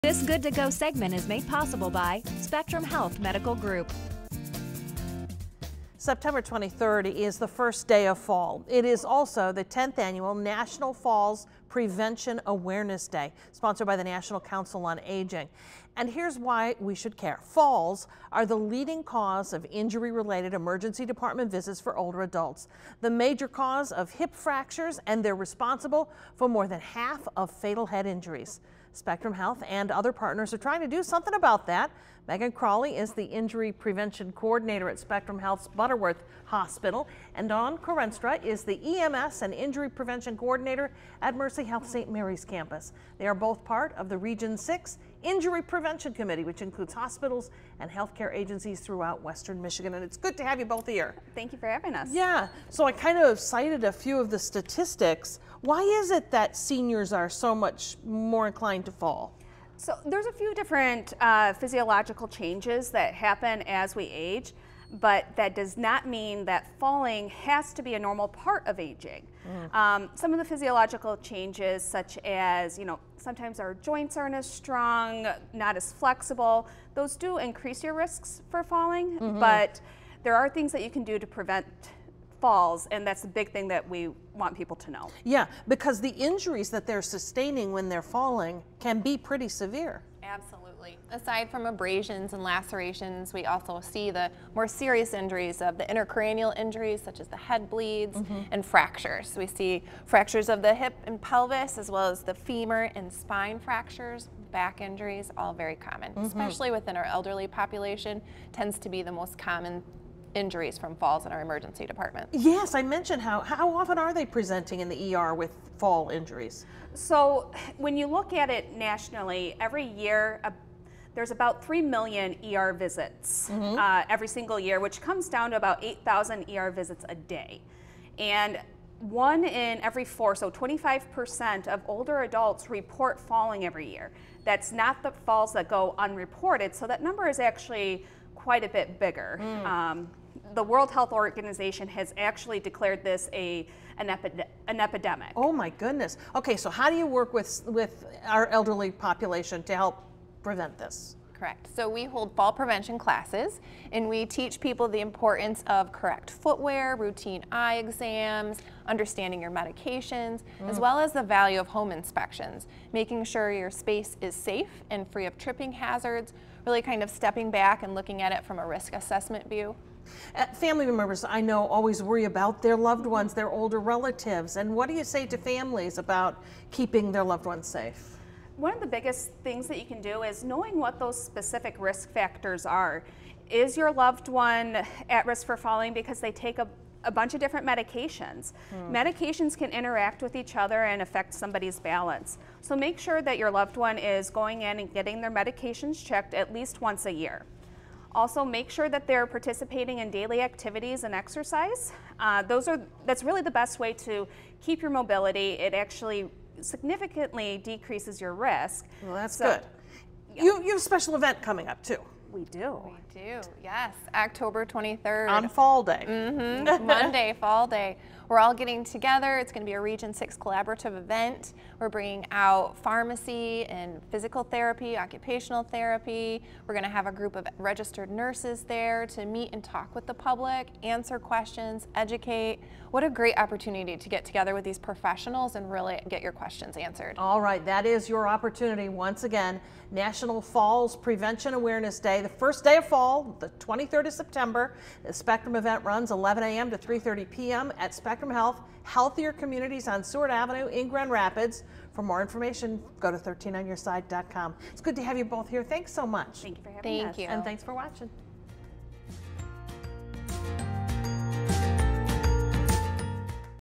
Good to Go: Spectrum Health
September 23 is the first day of fall and it also marks the start of the 10th annual National Falls Prevention Awareness Day. Spectrum Health is offering a special program to help prevent falls.